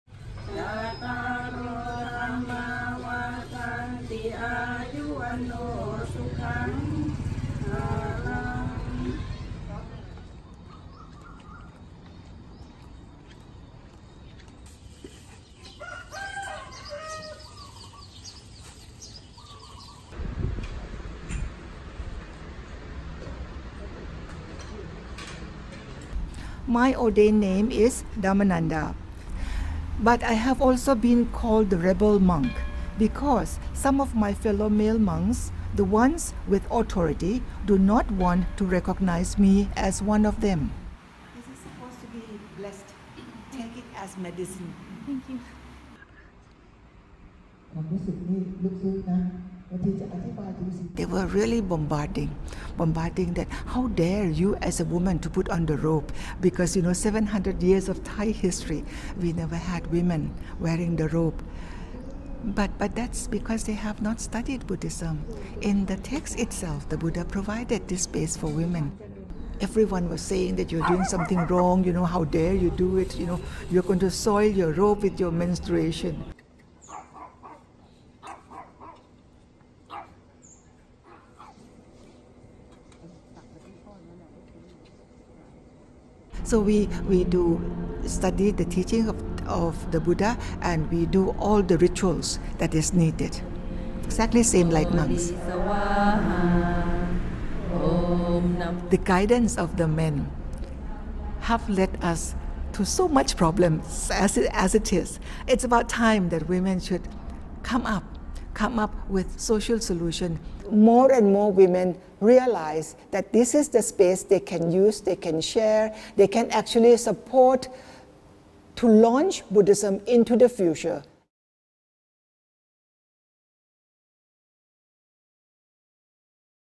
‘बंडखोर' महिला भिक्खूंनी का मोडली 700 वर्षांची परंपरा? | Thailand’s ‘rebel’ women monks
थायलंडमध्ये महिला भिक्खू होऊ शकत नाहीत अशी परंपरा आहे. पण काही महिला बंडखोरी करून भिक्खू झाल्या आहेत. परदेशात दीक्षा घेणाऱ्या धम्मनंदा या थायलंडच्या पहिल्या महिला भिक्खू आहेत.

टीका होऊनही त्यांनी महिलांसाठी विहार उभारलं आहे. थायलंडच्या 700 वर्षांच्या इतिहासात कोणीही महिला भिक्खू झालेली नाही. पण या महिलांनी करून दाखवलं. काय आहे त्यांची गोष्ट?